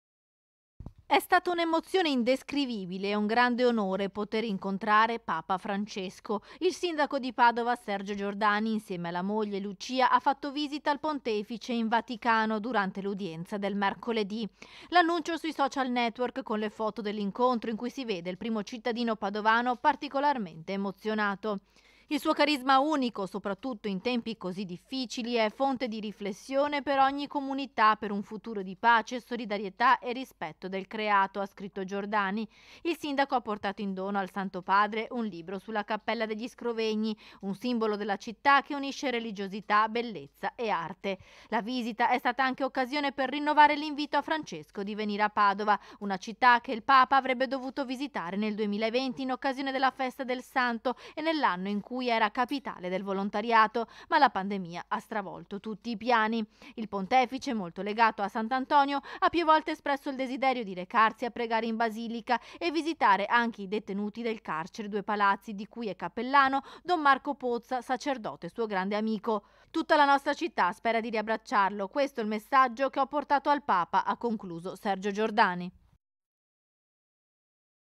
IL SINDACO GIORDANI IN VISITA DA PAPA FRANCESCO: «L'ASPETTIAMO A PADOVA» | 13/01/2022
TG Padova Sera 13/01/2022 - PADOVA - Durante l'udienza del mercoledì il sindaco di Padova Sergio Giordani ha incontrato Papa Francesco || "È stata un’emozione indescrivibile e un grande onore poter incontrare Papa Francesco" il sindaco di Padova Sergio Giordani insieme alla moglie Lucia ha fatto visita al Pontefice in Vaticano durante l'udienza del mercoledì. L'annuncio sui social network con le foto dell'incontro in cui si vede il primo cittadino padovano particolarmente emozionato. "Il suo carisma unico, soprattutto in tempi così difficili, è fonte di riflessione per ogni comunità per un futuro di pace, solidarietà e rispetto del creato" ha scritto Giordani. Il sindaco ha portato in dono al Santo Padre un libro sulla Cappella degli Scrovegni, un simbolo della città che unisce religiosità, bellezza e arte. La visita è stata anche l'occasione per rinnovare l'invito a Francesco di venire a Padova, una città che il Papa averebbe dovuto visitare nel 2020 in occasione della festa Santo e nell'anno in cui era capitale del volontariato, ma la pandemia ha stravolto tutti i piani. Il Pontefice molto legato a Sant'Antonio ha più volte espresso il desiderito di recarsi a pregare in Basilica e visitare anche i detenuti del carcere Due Palazzi di cui è capellano Don Marco Pozza sacerdote suo grande amico. "Tutta la nostra città spera di abbracciarlo questo il messaggio che ho portato al Papa" ha concluso Sergio Giordani. (Servizio di Valentina Visentin) - Segui Telenordest anche sul digitale terrestre! Visita il sito